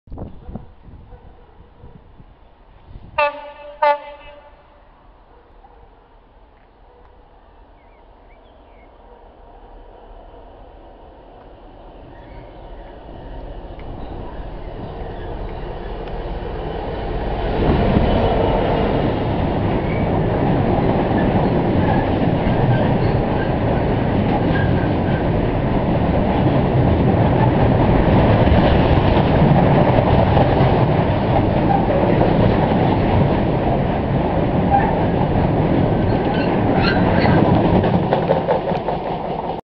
66615 passes through Heworth (2 tone)
Freightliner class 66 66615 passes through Heworth